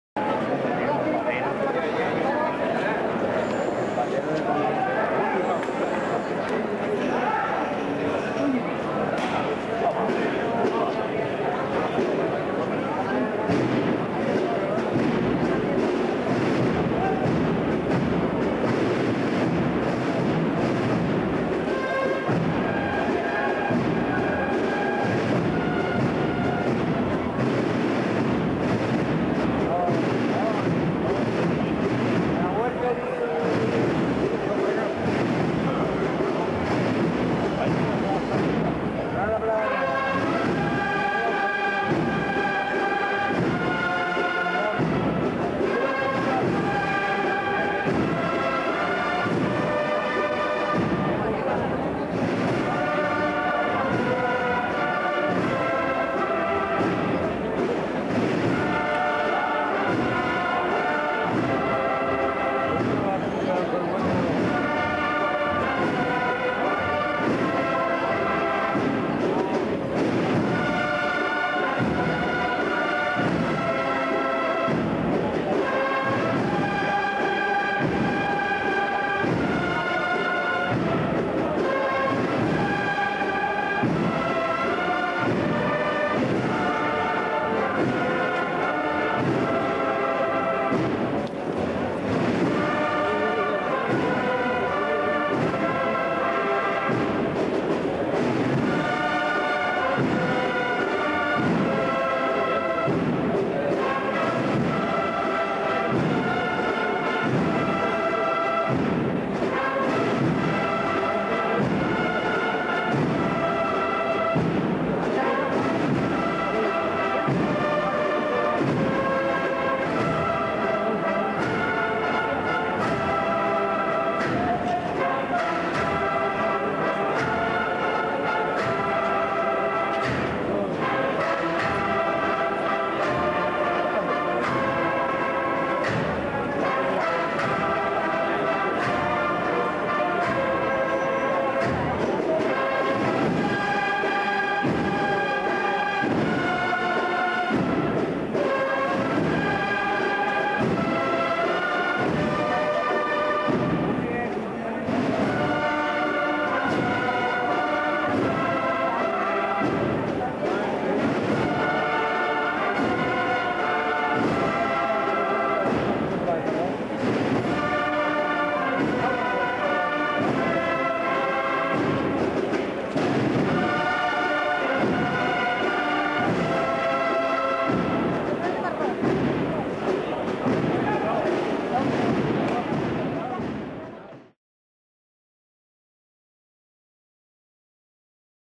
semana santa de sevilla 1990 - Jesús Despojado
Desfile del paso de Jesús Despojado por la calle Amor de Dios de Sevilla. Semana santa 1990